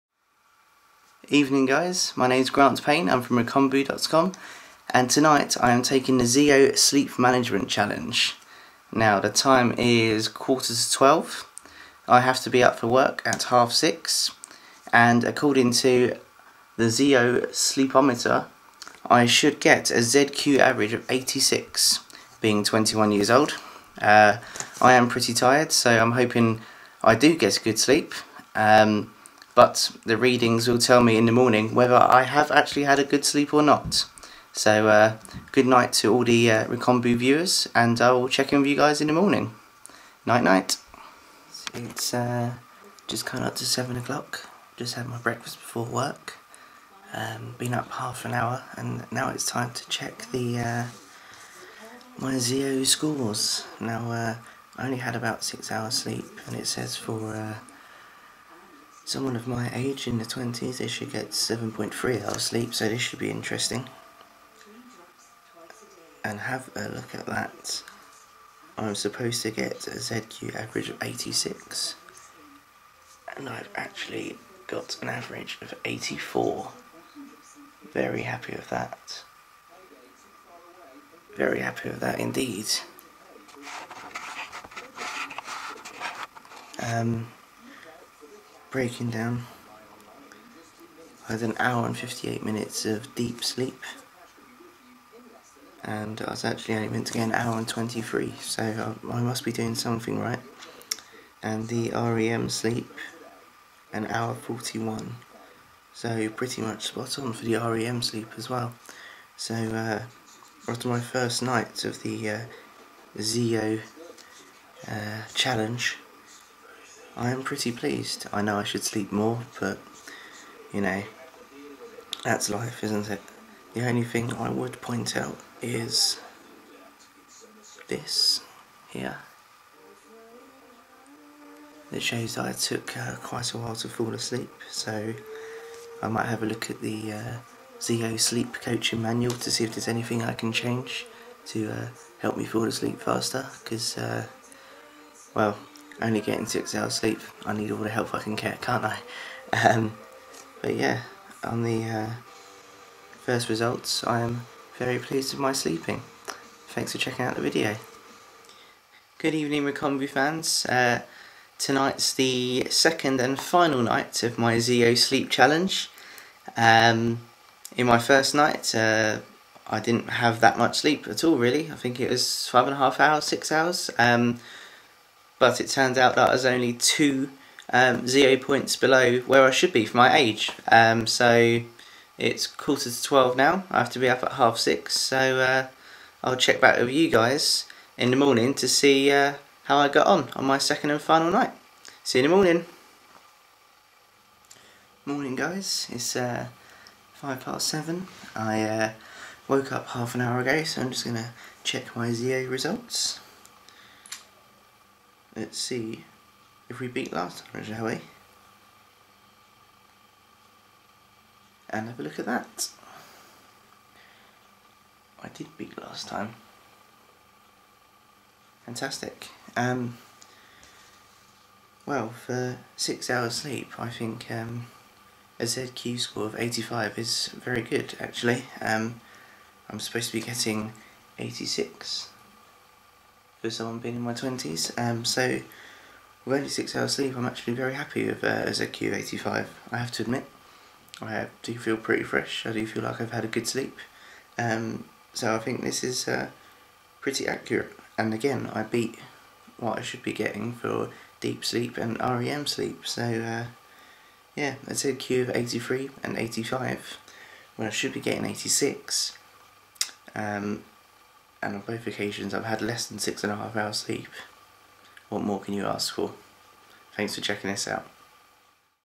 Zeo Sleep Management in action
Grant Payne takes the Zeo Sleep Management test - check out how he got on